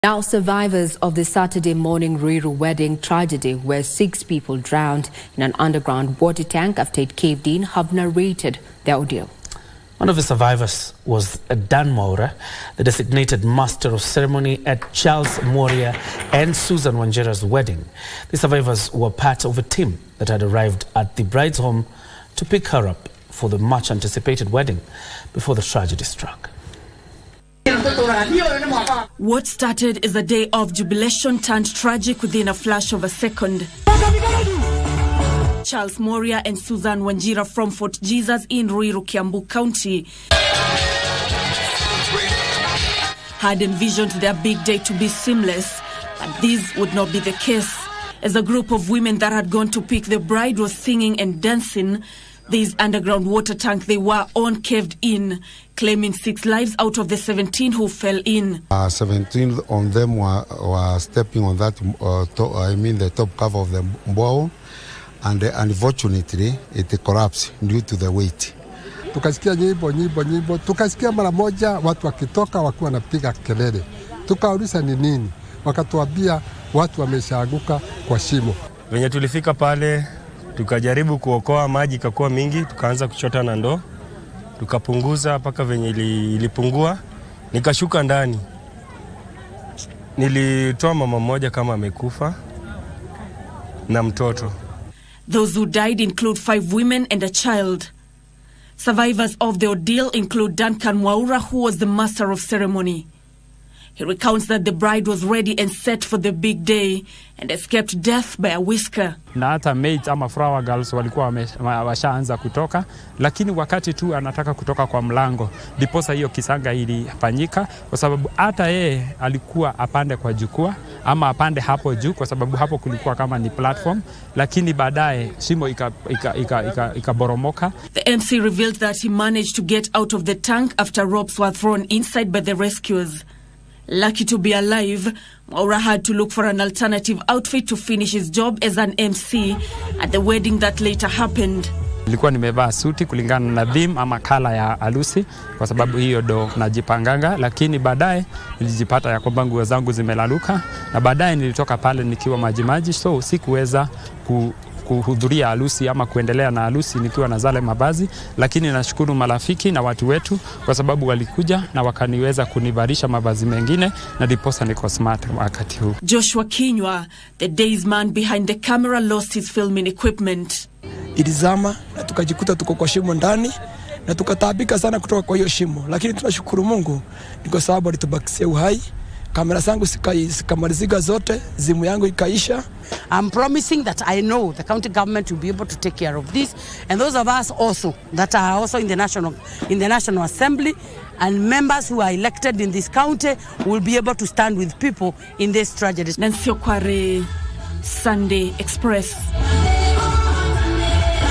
Survivors of the Ruiru wedding tragedy where 6 people drowned in an underground tank speak.
Survivors of the Saturday morning Ruiru wedding tragedy, where six people drowned in an underground water tank after it caved in, have narrated their ordeal. One of the survivors was Dan Mwaura, the designated Master of Ceremony at Charles Mworia and Susan Wanjira’s wedding.